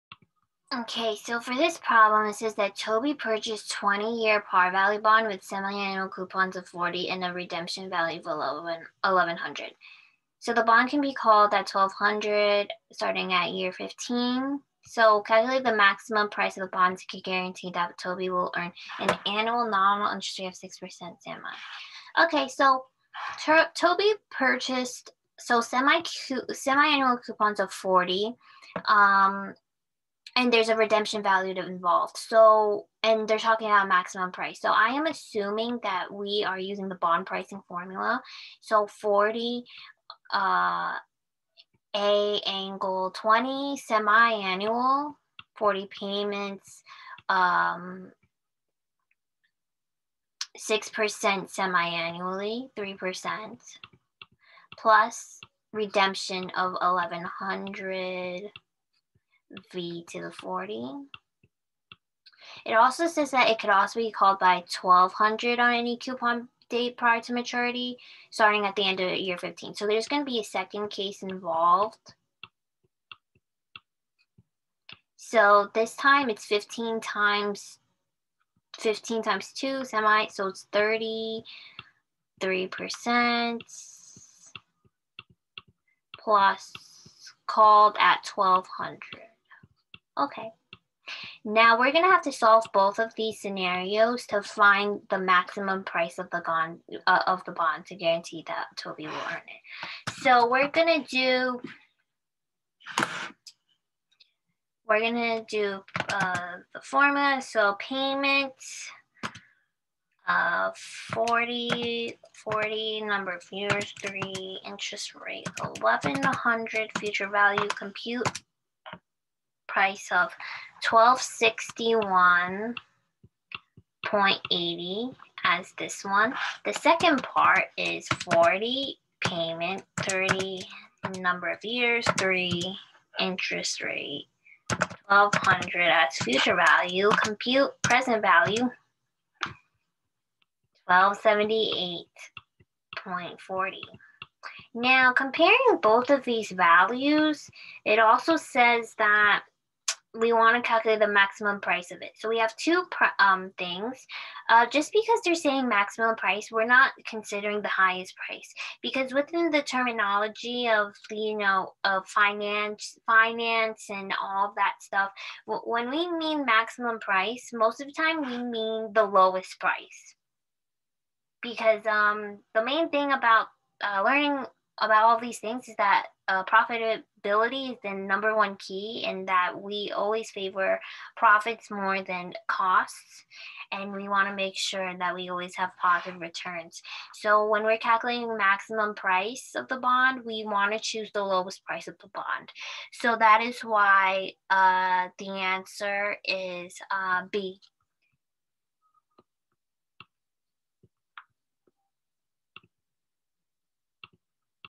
SOA/FM SAMPLE QUESTION #55
55. Toby purchased a 20-year par value bond with semiannual coupons of 40 and a redemption value of 1100. The bond can be called at 1200 on any coupon date prior to maturity, starting at the end of year 15.
Calculate the maximum price of the bond to guarantee that Toby will earn an annual nominal interest rate of at least 6% convertible semiannually.
(A) 1251
(B) 1262
(D) 1286
(E) 1295